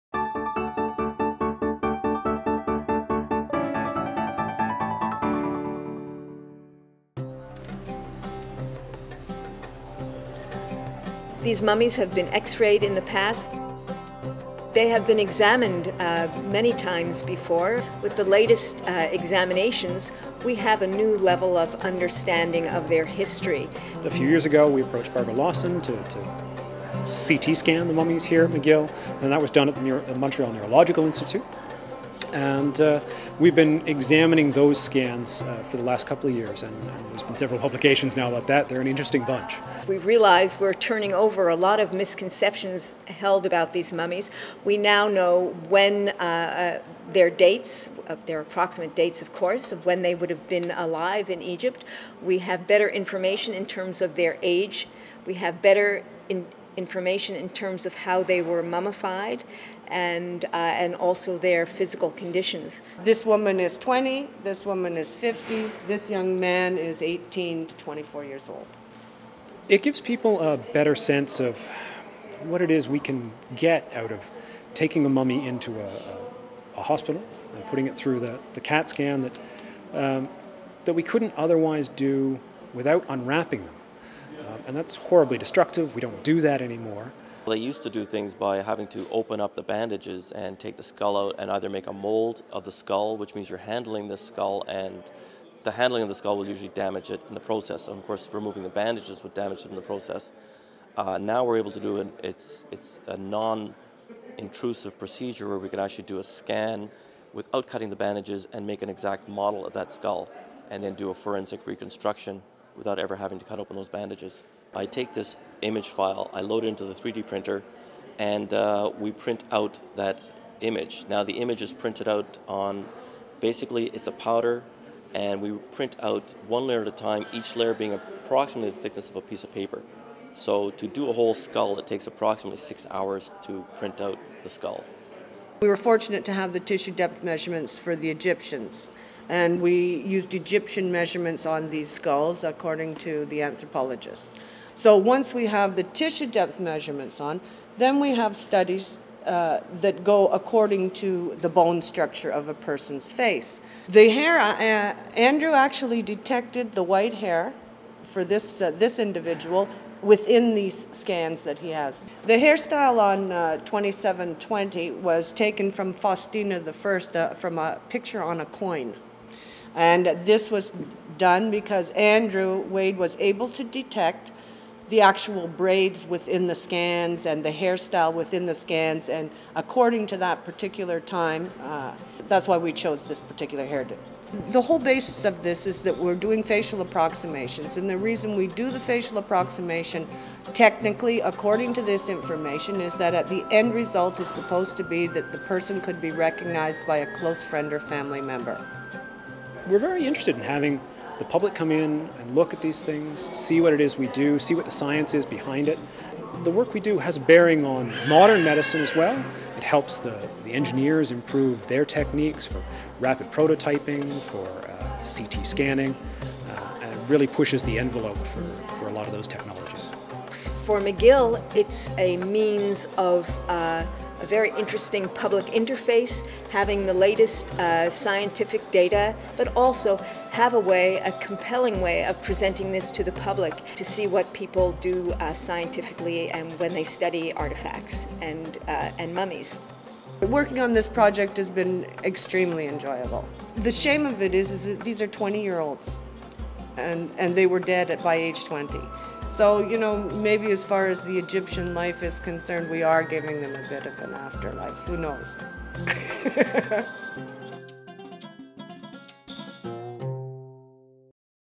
McGill Mummy Makeover
Three Egyptian mummies from McGill's Redpath Museum are "brought to life" thanks to facial reconstructions. Using skeletal data from recent CT scans and radiocarbon analyses, a forensic artist from John Abbott College and physical anthropologists from Western University were able to reconstruct the faces of the McGill mummies.